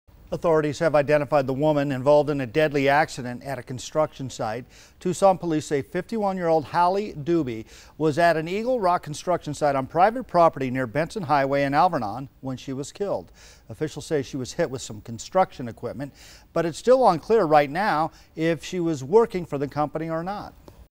Woman killed in construction accident on south side identified
51-year-old Hallie Dubey was killed in a construction accident near Benson Highway and Alvernon Way, according to Tucson Police Department.